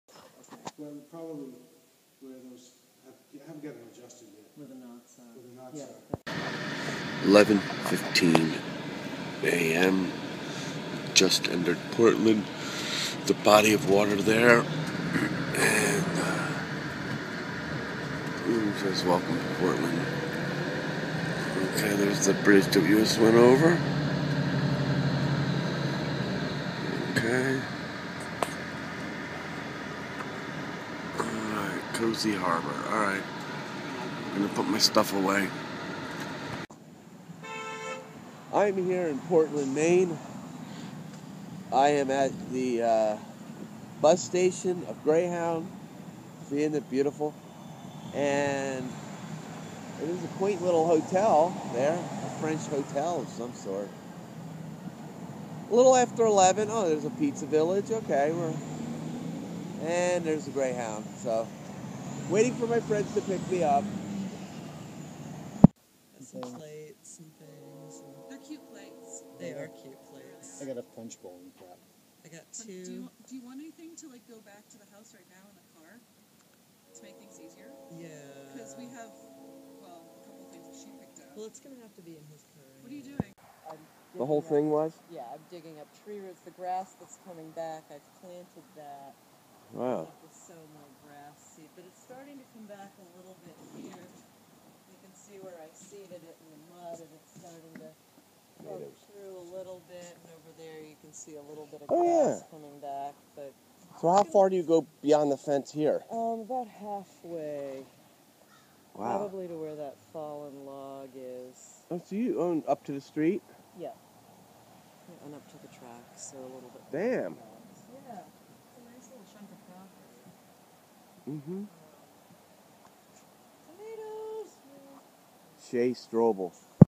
I Arrive in Portland Maine
It'a May 17th 2013 and after a 12 hour ride on Greyhound which took me to New York, Boston and then Portland. I was greeted by Tanya and her friend Cindy and prepare for Matt and Tany's Wedding